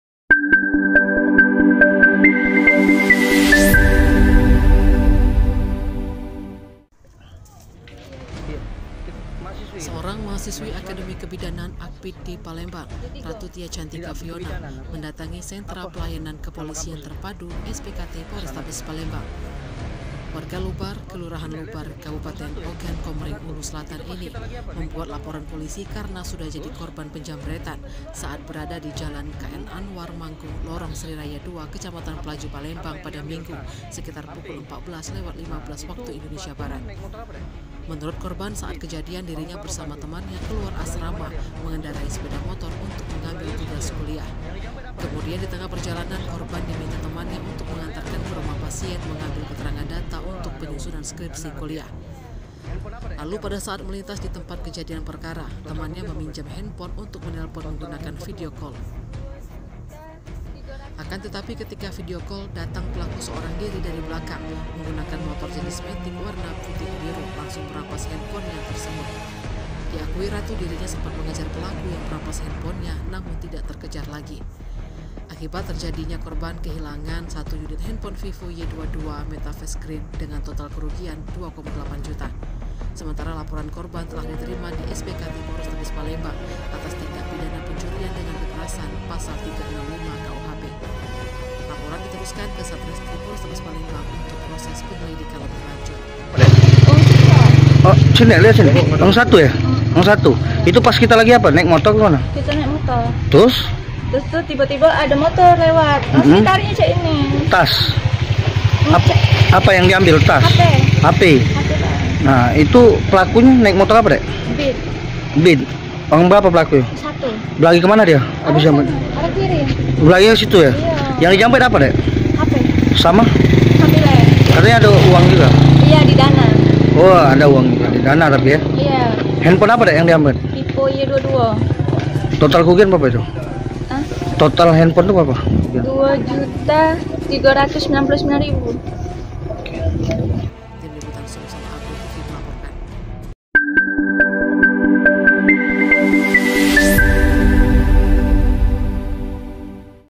Asyik Video Call, Handphone Milik Mahasiswi Akbid Palembang Ini Dirampas Penjambret | SumselUpdateTV
Palembang, Sumselupdate.com – Seorang mahasiswi Akademi Kebidanan (Akbid) di Palembang, Ratu Tya Cantika Viona (20) mendatangi Sentra Pelayanan Kepolisian Terpadu (SPKT) Polrestabes Palembang.

Warga Lubar, Kelurahan Lubar, Kabupaten Ogan Komering Ulu (OKU) Selatan ini, membuat laporan polisi, karena sudah jadi korban penjambretan saat berada di Jalan KI Anwar Mangku, Lorong Sri Raya…